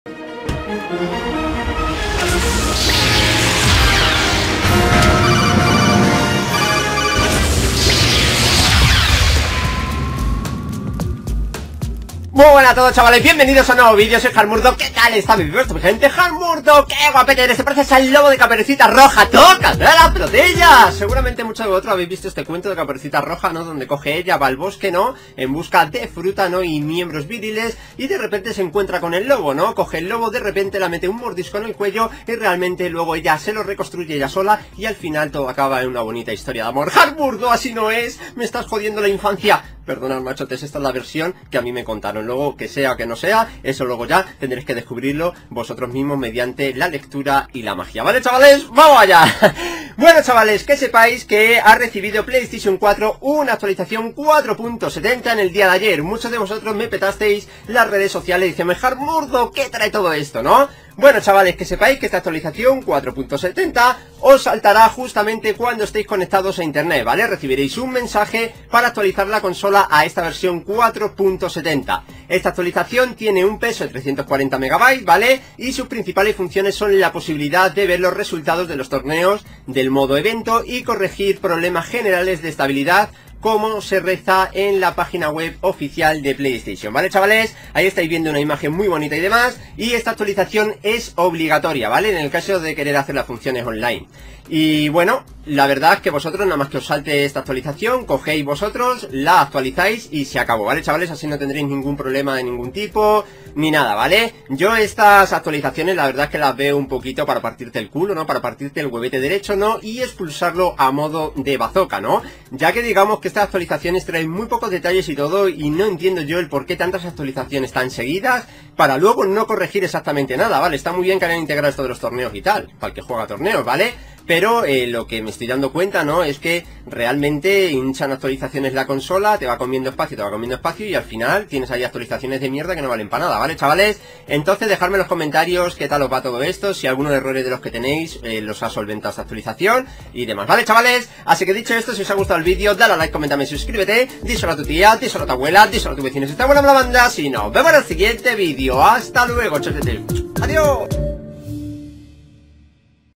¡¡¡QUÉ TRAE LA ACTUALIZACIÓN 4.70!!! Hardmurdog - Ps4 - Ps4 Pro - Sony - Playstation
✔ Aquél que inicie sesión en PlayStation 4 estando la misma conectada a internet, recibirá un mensaje para actualizar la consola a la versión 4.70. Esta actualización tiene un peso de 340MB y sus principales funciones son la posibilidad de ver los resultados de los torneos del modo evento y corregir problemas generales de estabilidad, como reza la web oficial de PlayStation.
✔ Si te ha gustado el vídeo díselo a tu tía, a tu abuela y a tu vecina...